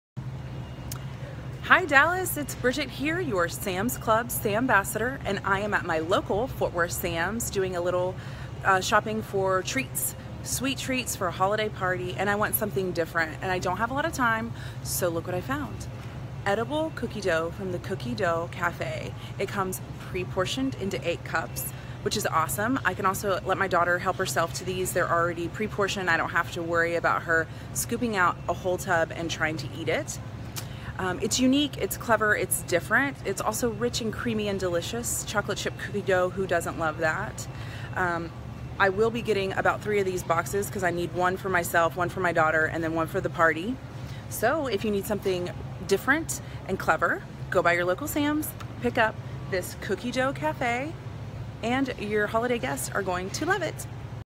🐽SNOUT OUT🐽 Cookie Dough
Bridgette shops for sweet treats at Sam's Club.  She finds pre-portioned edible cookie dough to bring to her next holiday party.